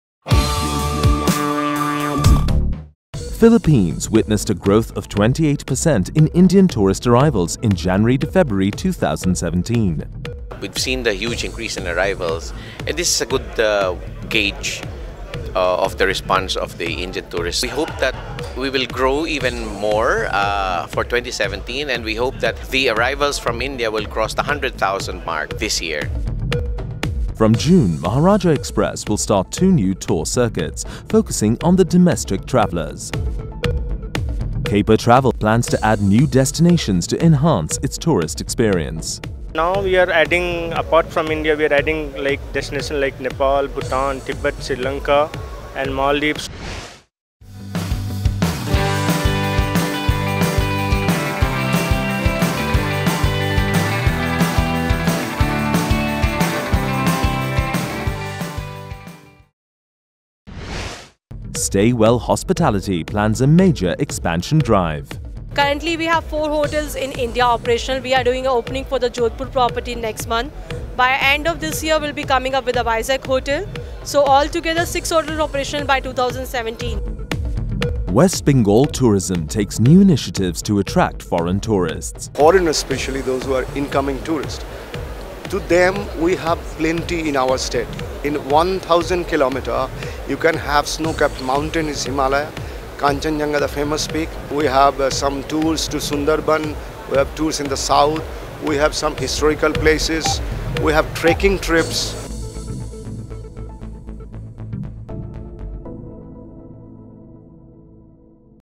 Traveltv.news Episode 442 (May 05)
In this episode TravelTV.News focuses on:

West Bengal Tourism takes new initiatives to attract foreign tourists
From June, Maharaja Express will start Two new tour circuits focusing on the Domestic Travelers
Caper Travel plans to add new destinations to enhance its tourist experience
Philippines witnessed a growth of 28% in Indian tourist arrivals in January-February, 2017
Stay well hospitality plans a major expansion drive
We interviewed:

GLEN AGUSTIN, Chief Tourism Operations Officer Market Development Group, Department of Tourism Philippines
BABLU KUMAR DAS, Tourist Officer-Dept of Tourism , Government of West Bengal
VISHAL JAISWAL, CEO, Caper Travel
SURBHI VOHRA, Regional Director, Stay Well Hospitality